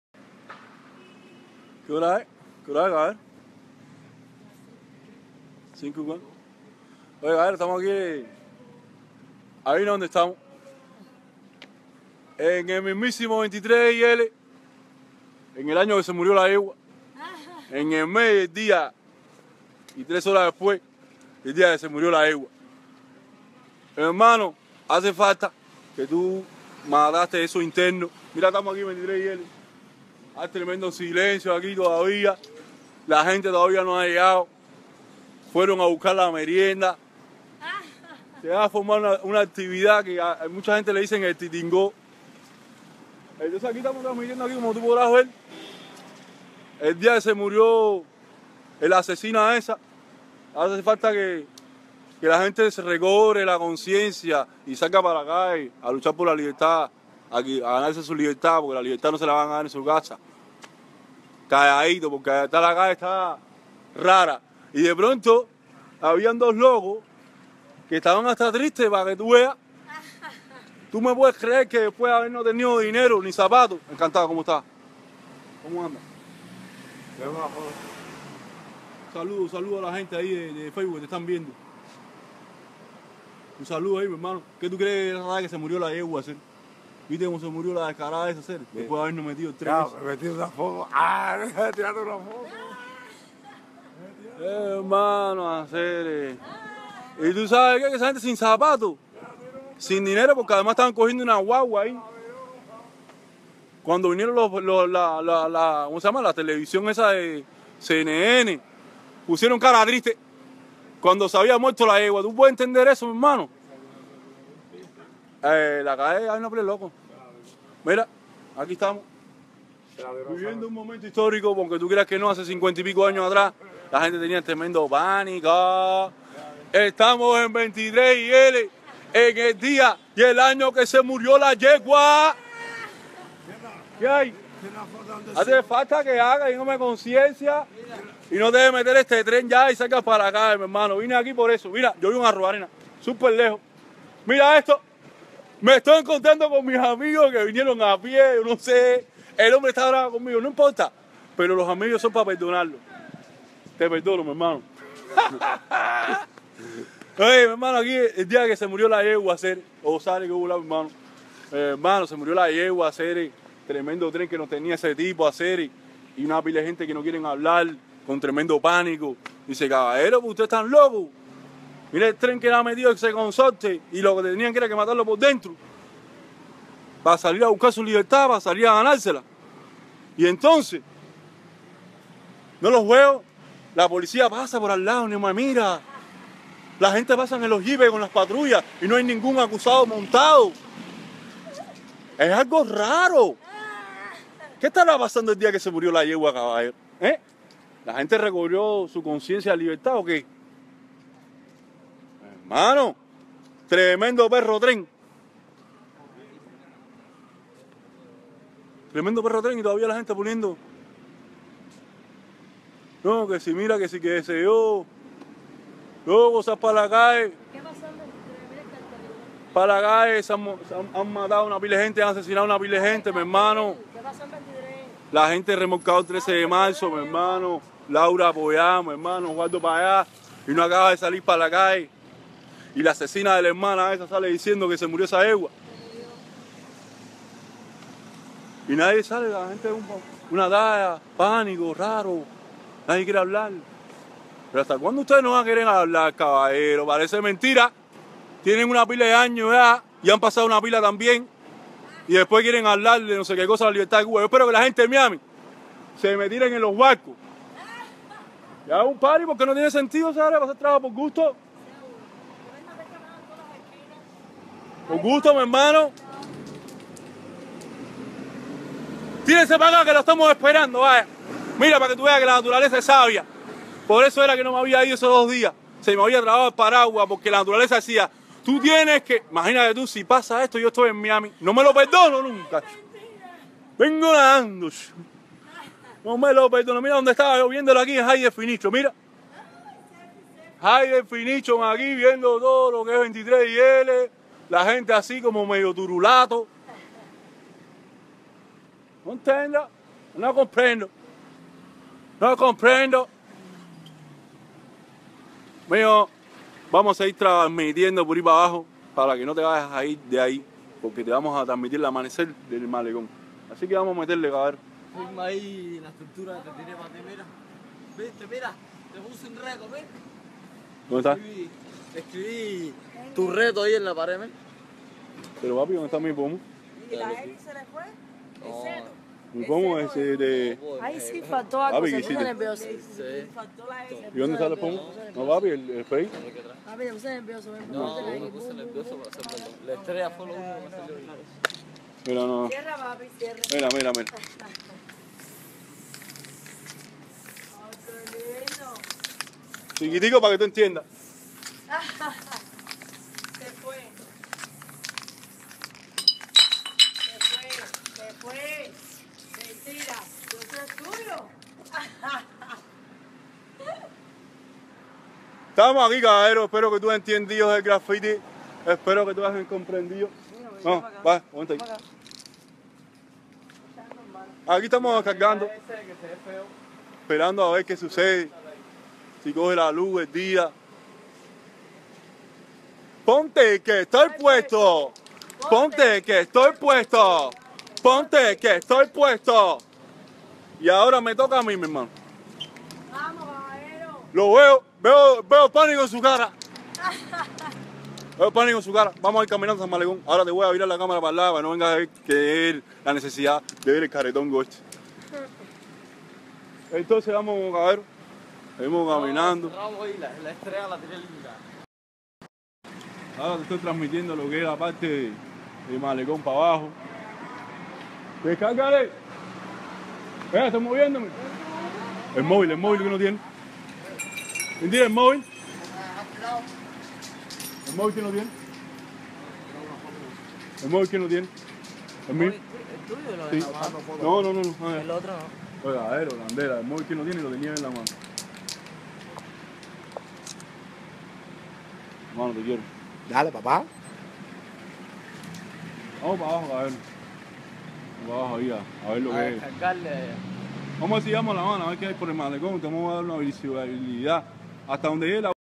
Se fue Fidel.
El día que Fidel Castro murió en Cuba, grafitiando, Se fue en la pared de Habana Libre antiguo Habana Hilton, en el Vedado,C habana,Cuba. en la madrugada del 26 de noviembre del año 2016.